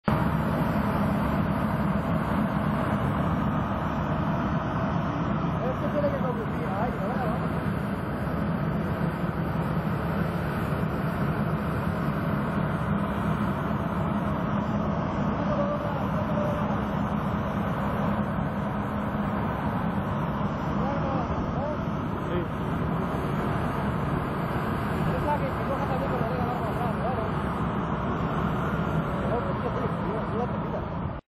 Aplicador de Fitosanitarios Cañon suspendido Agrosan
El chasis fabricado en perfilería y chapa de acero de alta calidad, reforzado con pintura termoendurecida que confiere unas caracteristicas adecuadas para la utilización del equipo en condiciones climáticas adversas y a la interperie. La geometría ha sido cocebida para garantizar una gran estabilidad del conjunto, asi como los diversos esfuerzos a los que se verá sometida la estructura.